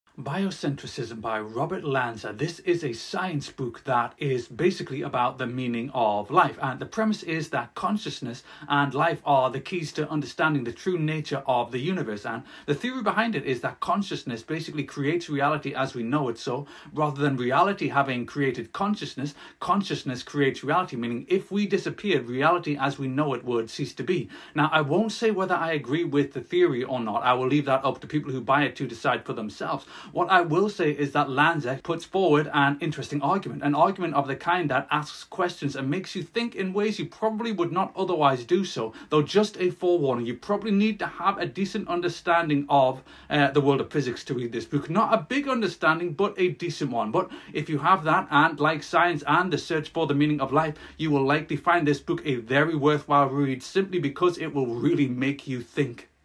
Biocentrism by Robert Lanza – a Great Read and Will Really Make You Think
A book review of Biocentrism by Robert Lanza – which is a Great Read and Will Really Make You Think.